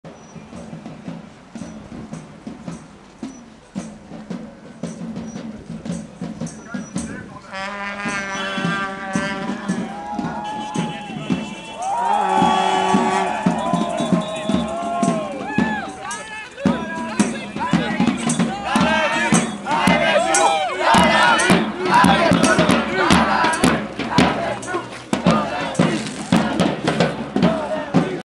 10e manif de nuit
8 mai 2012, manif étudiante de nuit à Québec sur René-Lévesque.